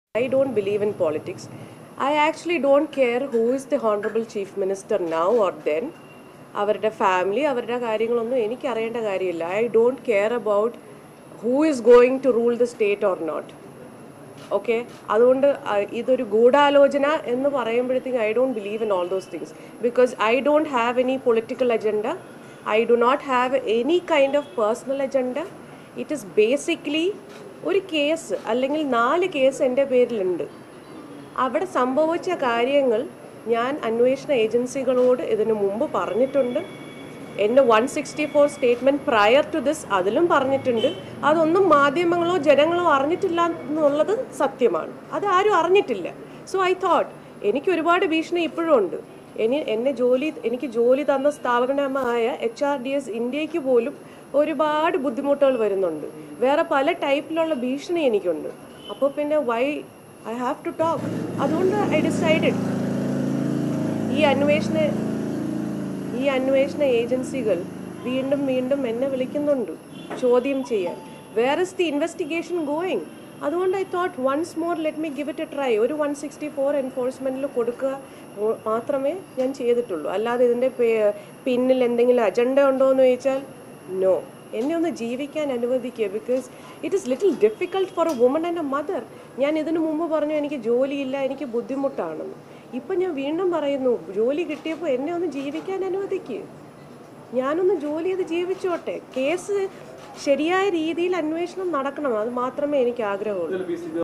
'എനിക്ക് രാഷ്ട്രീയ അജണ്ടയില്ല, എന്നെ ജീവിക്കാന്‍ അനുവദിക്കൂ' | Mathrubhumi News
തനിക്ക് രാഷ്ട്രീയമില്ലെന്നും അന്നത്തെ മുഖ്യമന്ത്രിയാരാണെന്നോ ഇന്നത്തെ മുഖ്യമന്ത്രി ആരാണെന്നോ  തനിക്ക് അറിയേണ്ട കാര്യമില്ലെന്നും സ്വപ്‌ന സുരേഷ് മാധ്യമങ്ങളോട്

Happy viewing!